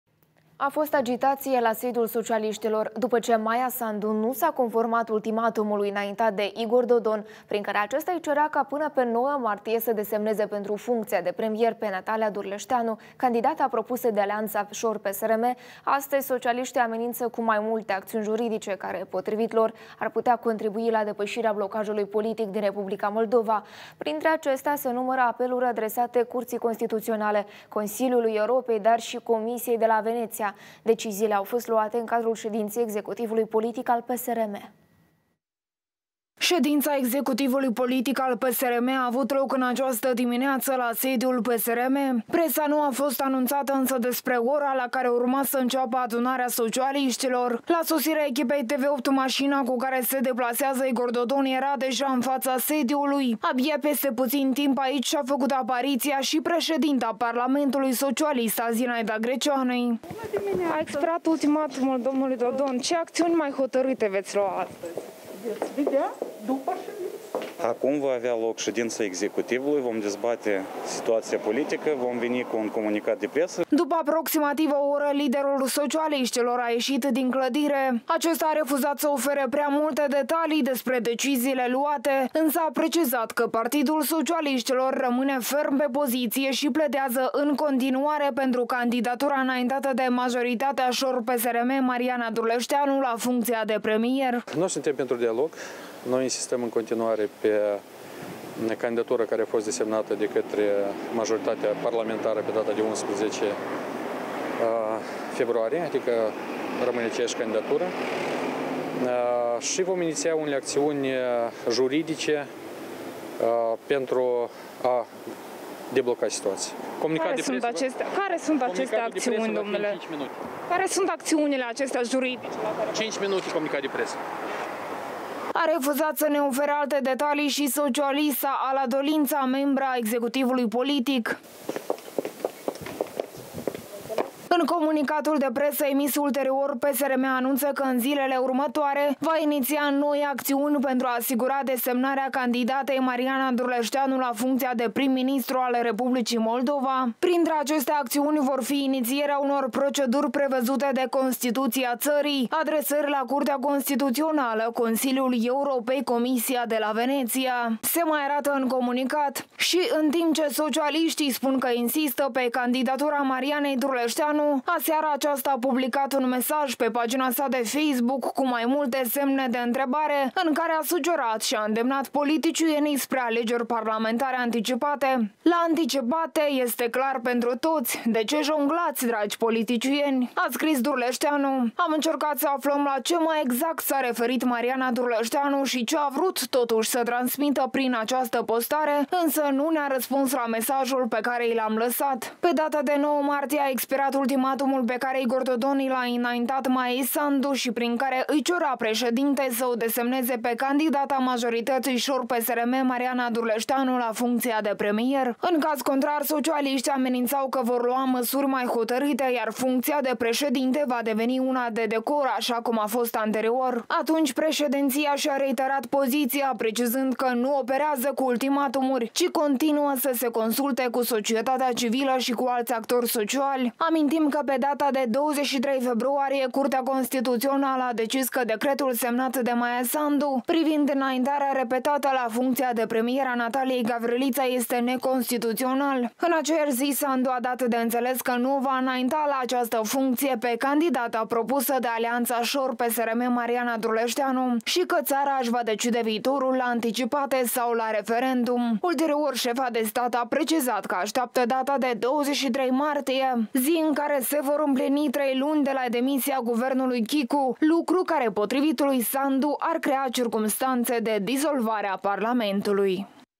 PSRM amenință cu „acțiuni juridice”
A fost agitație, la sediul socialiștilor. După ce Maia Sandu nu sa conformat ultimatumului înaintat de Igor Dodon, prin care acesta îi cerea ca, până pe 9 martie să o desemneze pentru funcția de premier pe Natalia Durleșteanu, candidata propusă de alianța ȘOR - PSRM, astăzi, socialiștii amenință cu mai multe acțiuni juridice care, potrivit lor, ar putea contribui la depășirea „blocajului politic” din Republica Moldova. Printre acestea se numără apeluri adresate Curții Constituționale, Consiliului Europei, dar și Comisiei de la Veneția. Deciziile au fost luate în cadrul ședinței Executivului Politic al PSRM.